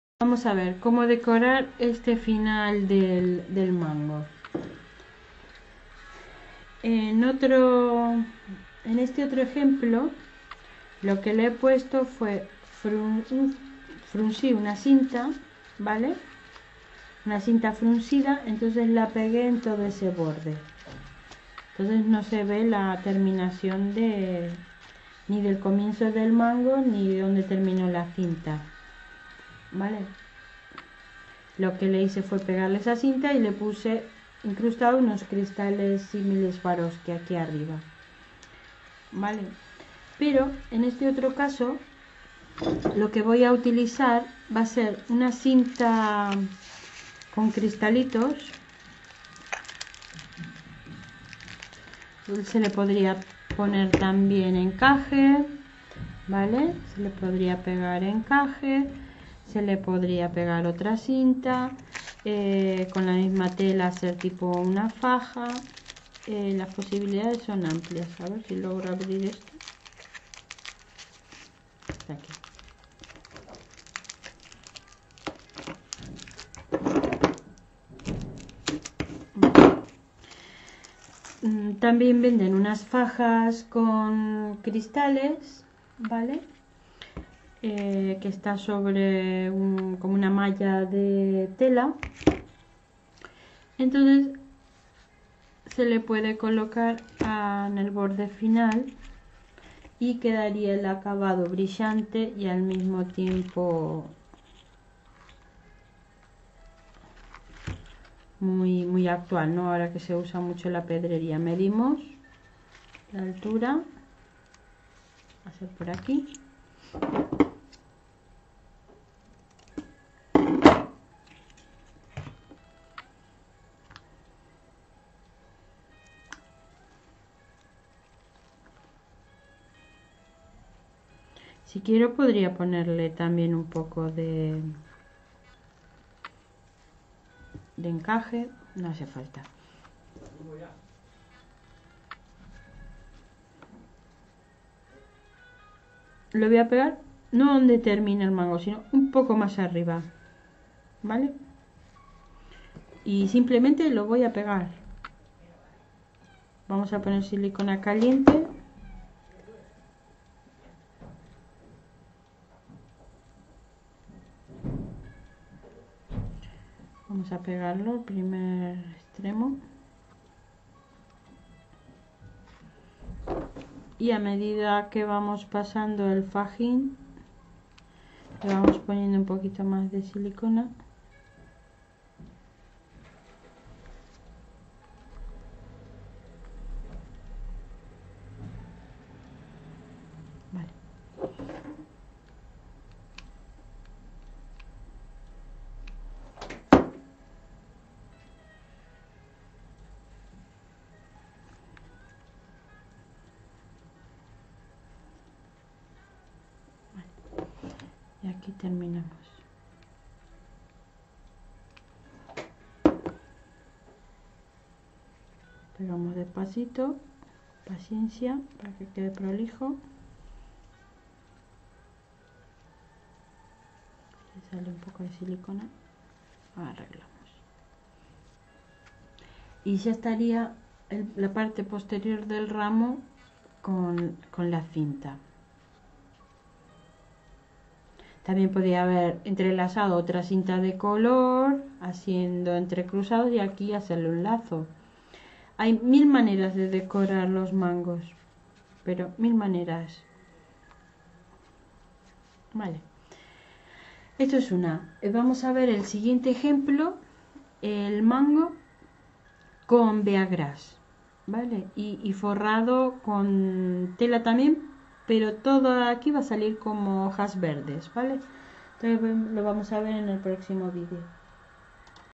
Hacemos una Rosmelia 1M Estructura de alambre para version 1 forrar mango Video 198
Como se hace un Ramo de Novia Rosmelia, este tutorial es para hacer una Rosmelia con flor preservada, de organza y petalo artificial, no con flor natural fresca
si quieres ver el curso completo tienes que dirigirte a la lista de reproduccion en este enlace, asi podras ver el orden correcto de los videos gracias